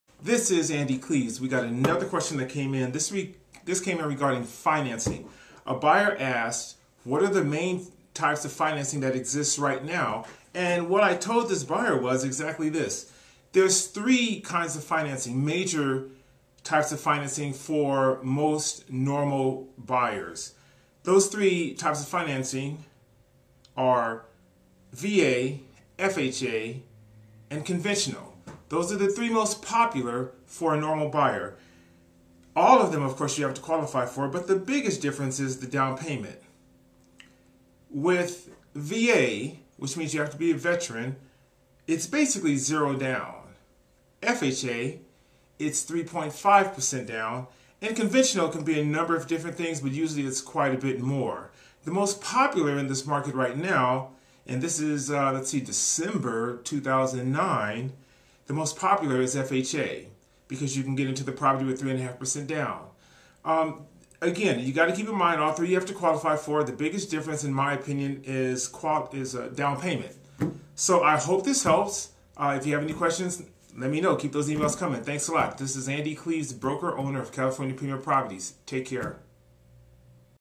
Andy Cleaves Financing
Andy talks about popular types of real estate financing for most buyers in this market.